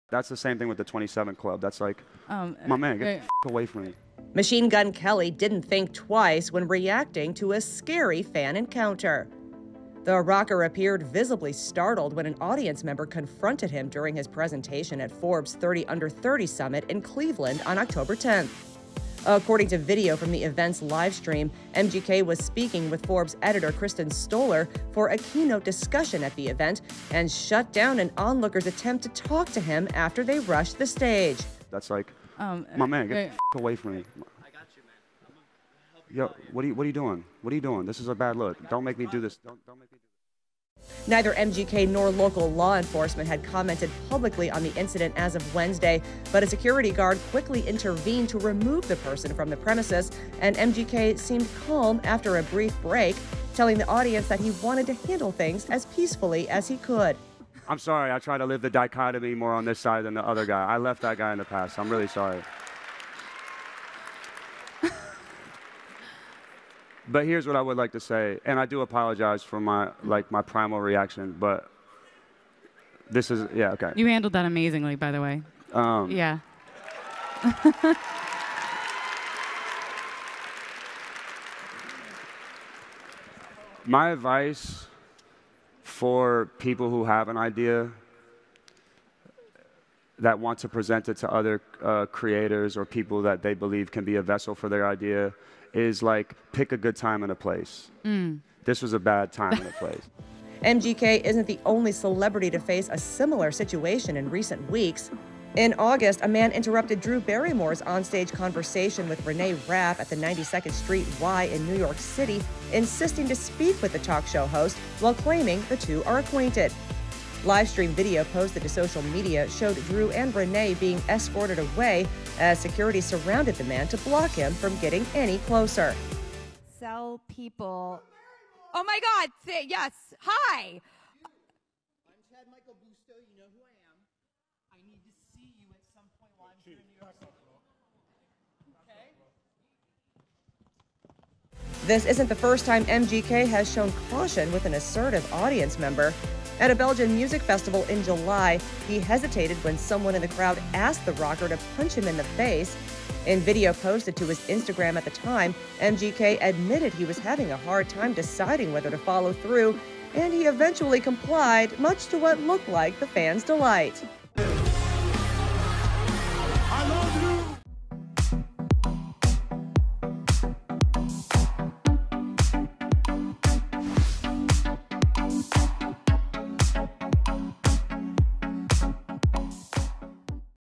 Machine Gun Kelly JUMPS When Fan Rushes Stage At Live Event
Machine Gun Kelly didn't think twice when reacting to a scary fan encounter. The rocker was visibly startled when an audience member confronted him during his presentation at Forbes' 30 Under 30 Summit in Cleveland on Oct. 10. According to fan-captured video from the event's livestream, MGK was speaking with Forbes editor Kristin Stoller for a keynote discussion at the event and shut down an onlooker's attempt to talk to him after they rushed the stage. "My man, get the f*** away from me. Yo, what are you doing? What are you doing? This is a bad look. Don't make me do this," MGK said, before a security guard removed the person from the premises. Neither MGK nor local law enforcement commented publicly on the incident as of Wednesday but the musician appeared to shake it off after a brief break, telling the audience that he wanted to handle things as peacefully as he could. "I'm sorry, I try to live the dichotomy more on this side than the other guy," he said with a smile. "I left that guy in the past, I'm really sorry."